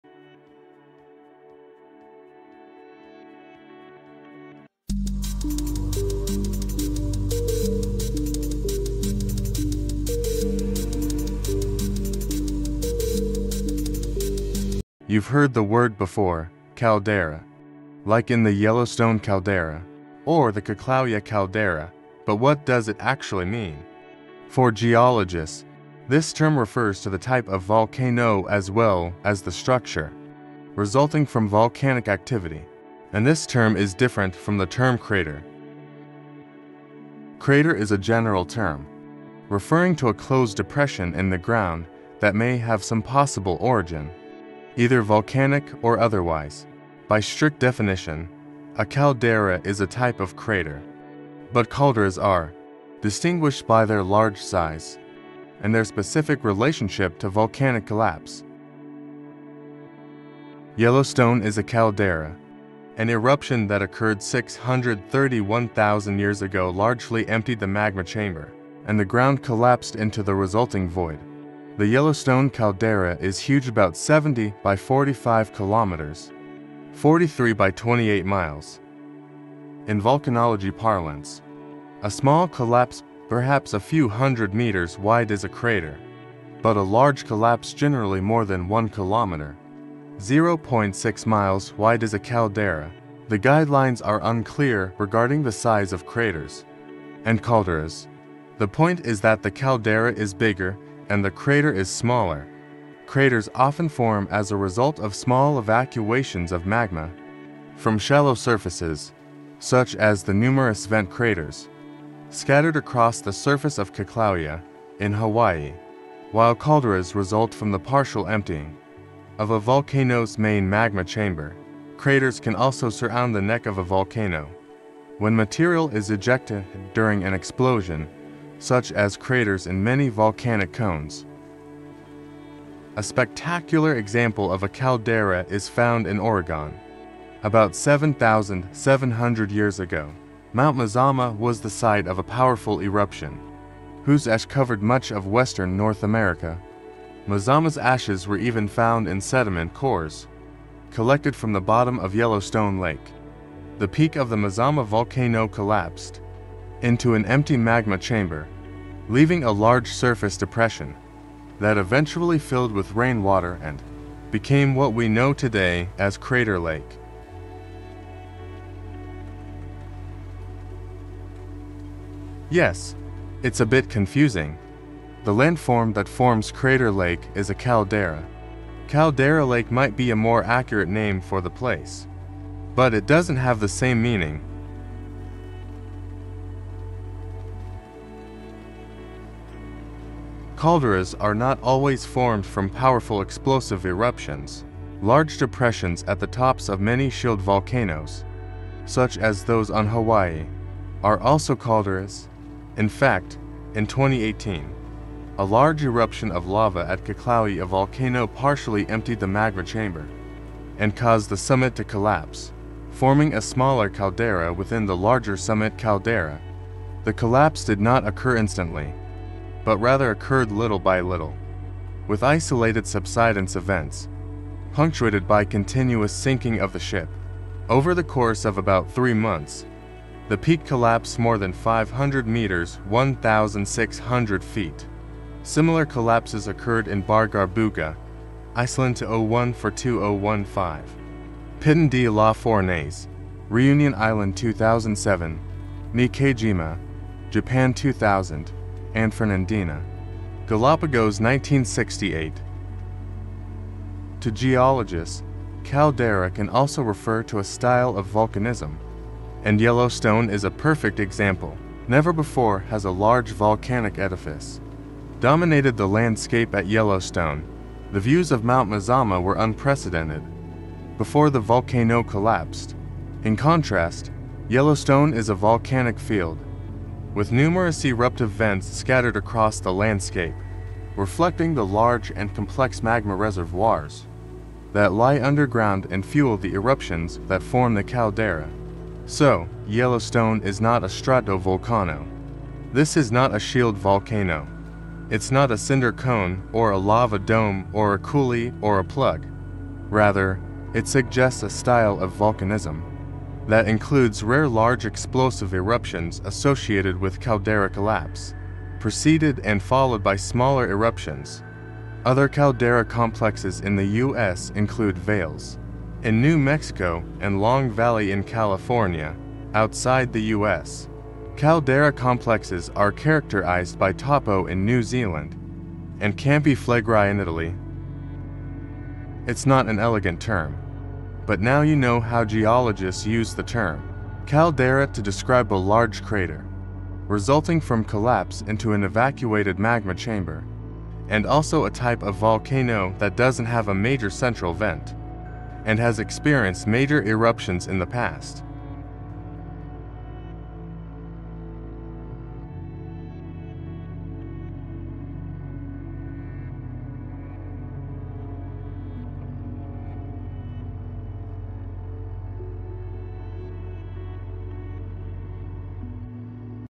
Horrible:Yellowstone Crater Collapses, USGS sends New Explosion warning Increases Threat to Millions
Horrible Today: Yellowstone Crater Collapses, USGS sends New Explosion warning Increases Threat to Millions

An eruption that occurred 631,000 years ago largely emptied the magma chamber, and the ground collapsed into the resulting void. The Yellowstone Caldera is huge—about 70 by 45 kilometers (43 by 28 miles).

The Yellowstone National Park area has experienced three major eruptions throughout the history of the earth. The largest eruption that formed the caldera occurred 2.1 million years ago and resulted in 2,450 cubic kilometers of material being ejected, giving it super volcano status.